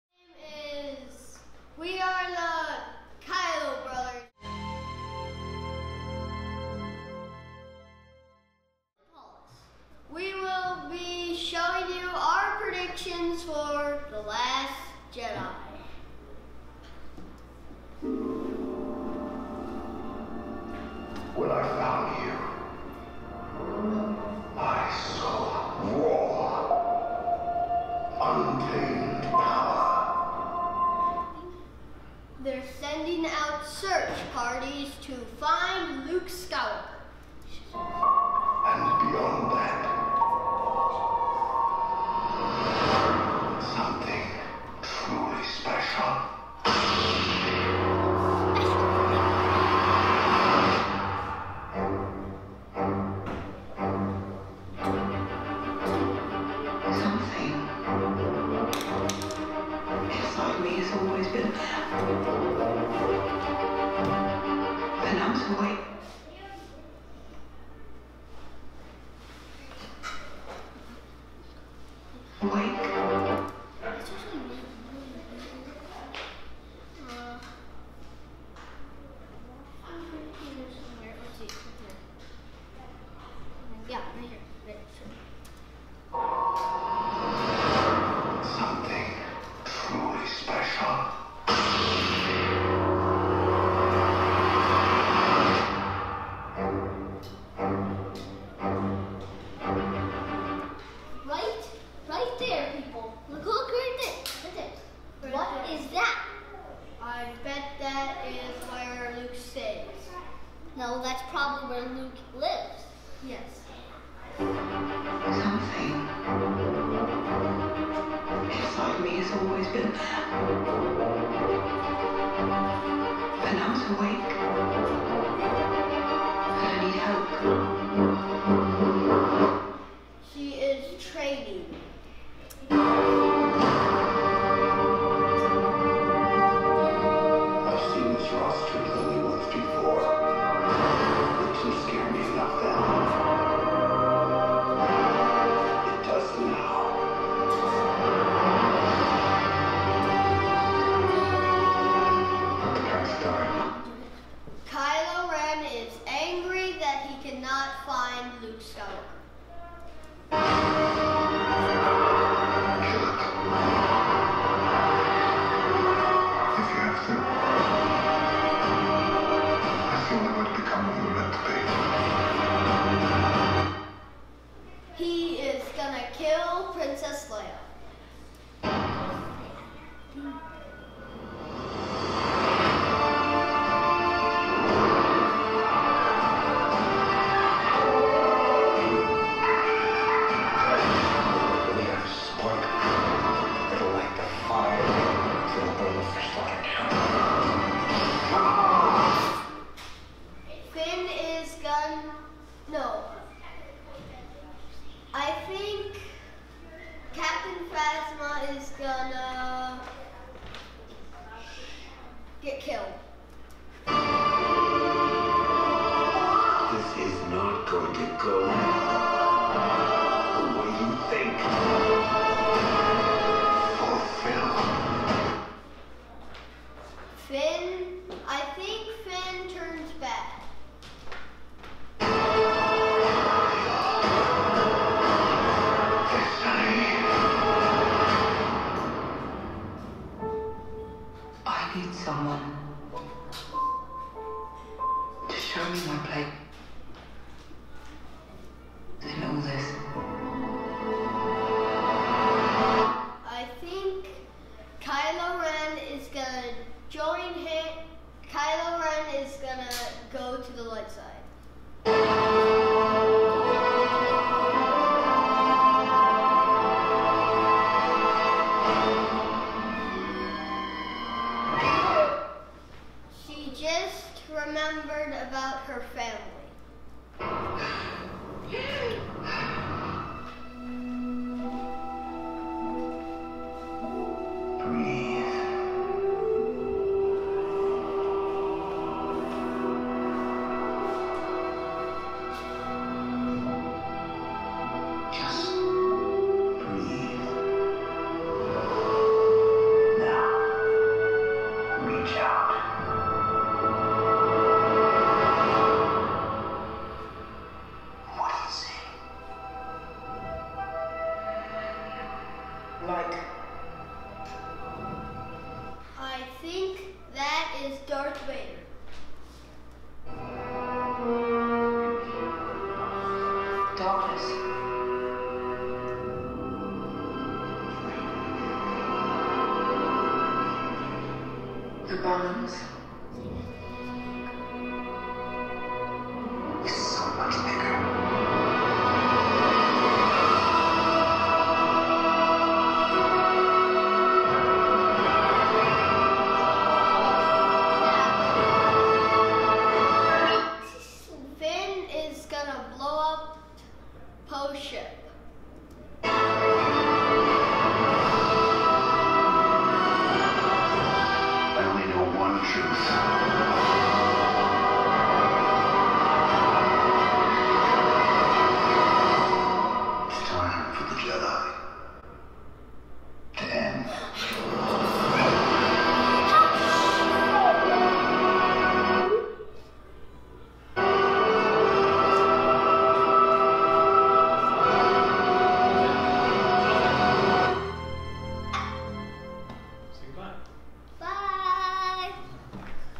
Last Jedi Predictions: The Kylo Brothers
Part of a video series project by Riverside Tutorial. Various teams prognosticate what will occur in the next Star Wars Film, The Last Jedi. Taking hints from the trailers, can they predict the story points?